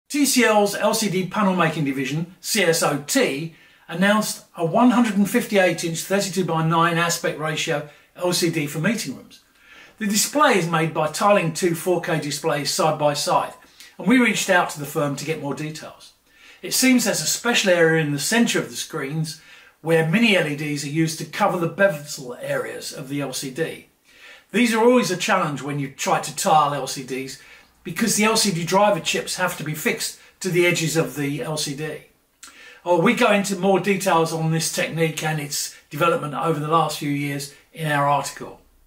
TCL CSOT Reveals More on 158" LCD
In a recent press release, the panel making division of TCL, the CSOT group, announced a 158″ LCD for ‘blackboard’ use that has been shown in Chia. We reached out to the firm for more details as it has historically been very difficult to create LCDs over 110″ to 120″ (for technical readers, this is related to the difficulty of driving the pixels efficiently over the relatively long electrodes from the edge of the panel).